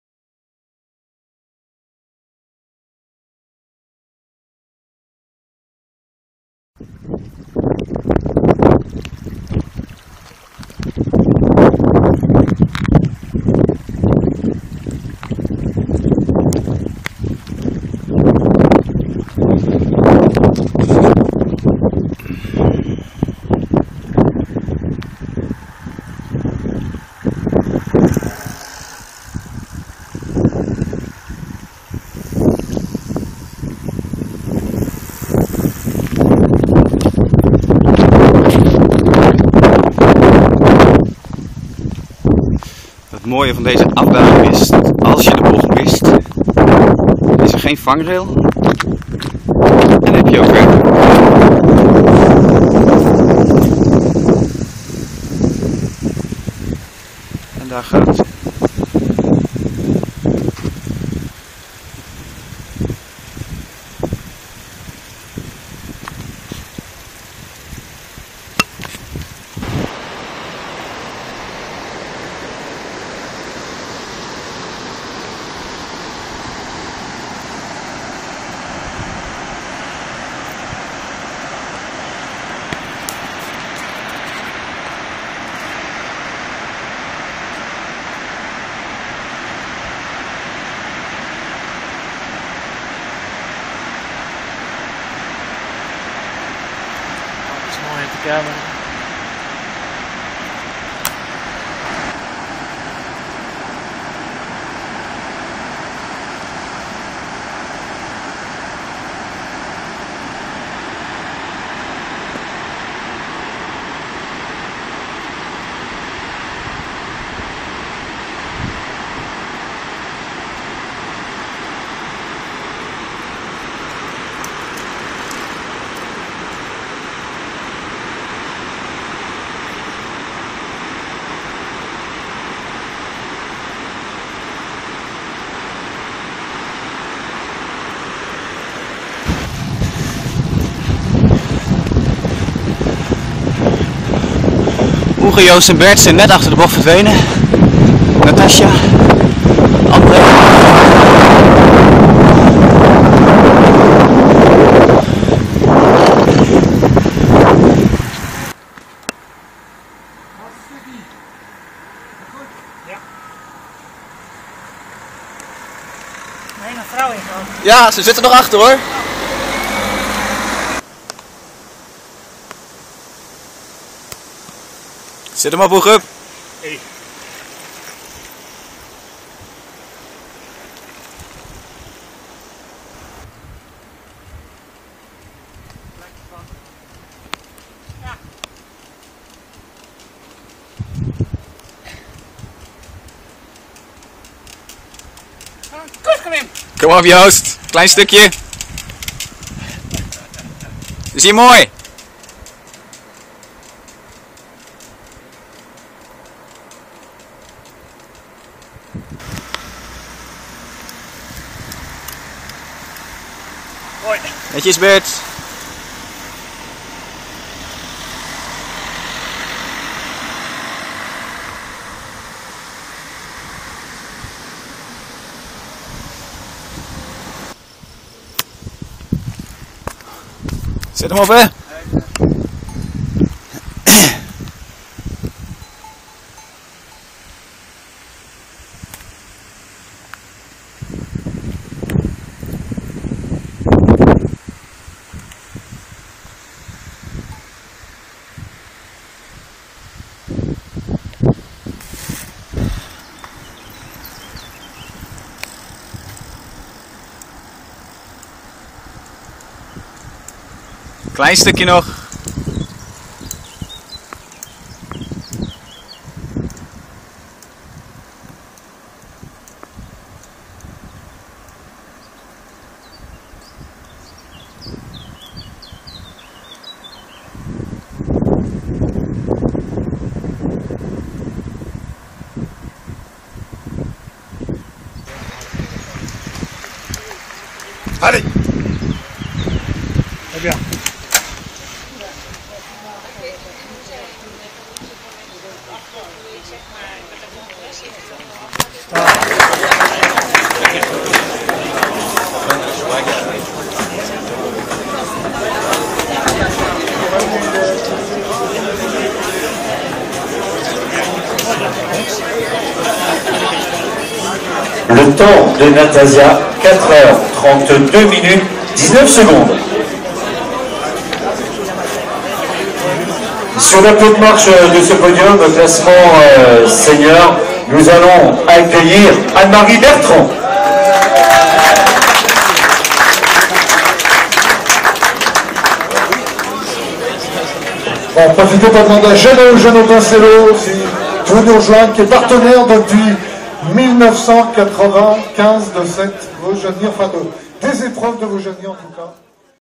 Impressie Vaujany
Wat korte filmfragmenten van de Vaujany achter elkaar gezet. De Vaujany is een cyclo waar in 2013 een grote groep cyclesporters succesvol aan deelnam. Op de korte afstand werd Natasja van der Zwaag 2e overall en in haar leeftijdscategorie. Op de lange afstand werd Oege Hiddema 10e overall en 2e in zijn leeftijdscategorie. Op die afstand wisten maar liefst 5 cyclesporters bij de eerste 50 overall te finishen!